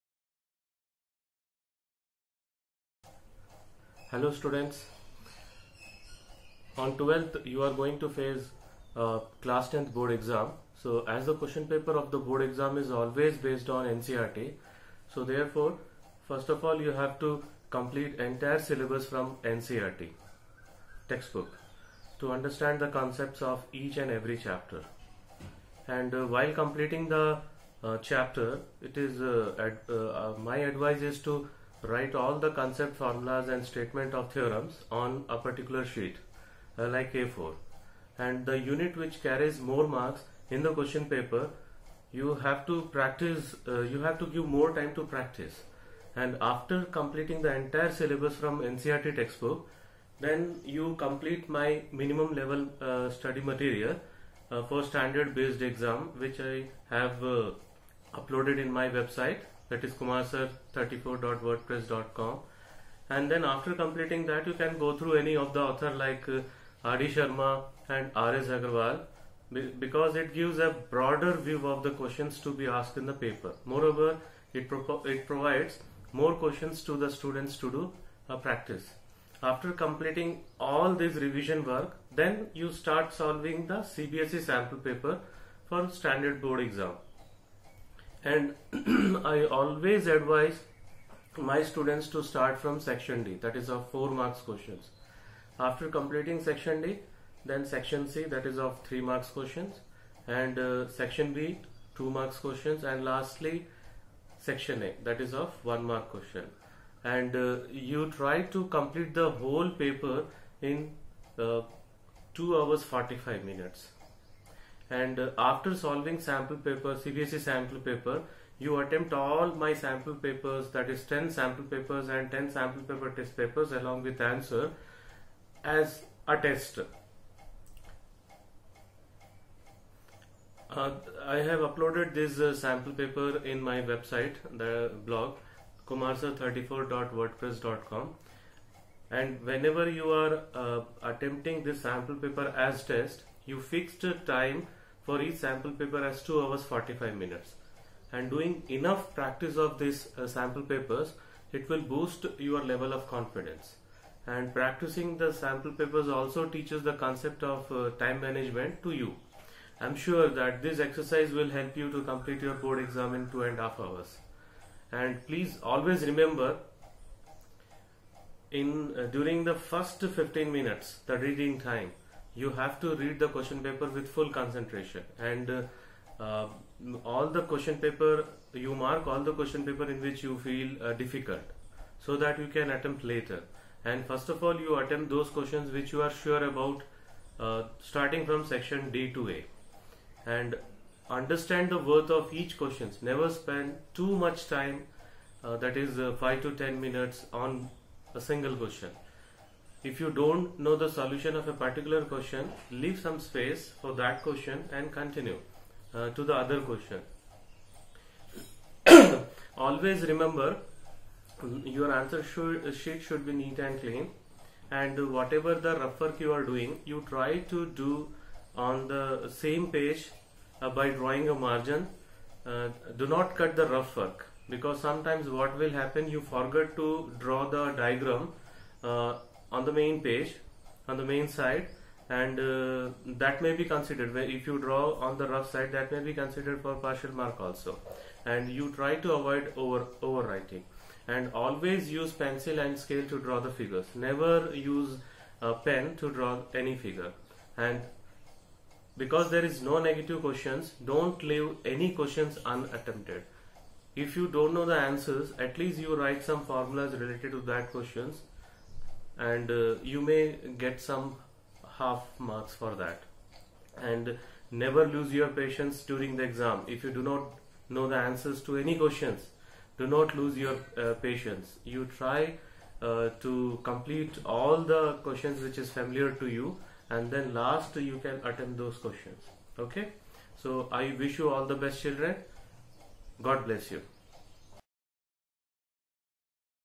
Tips for Class 10 Maths Board Exam
My tips for Class 10 Maths Standard Board Exam. Don't leave any question un-attempted. Write down anything you know about the question even if it is wrong. There is no negative marking so attempting a question, even if you don't know the answer completely, will not hurt.